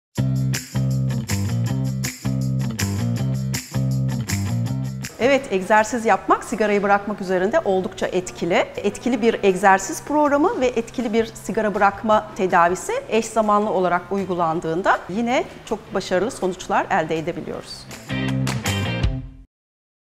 Spor, Sigarayı Bırakmaya Yardımcı Olur Mu? - Uzm. Dr. Hacer Ofluoğlu
Medicana Çamlıca Hastanesi Göğüs Hastalıkları bölümünden Uzm. Dr. Hacer Ofluoğlu ''Spor, Sigarayı Bırakmaya Yardımcı Olur Mu?'' sorusunu sizler için yanıtladı.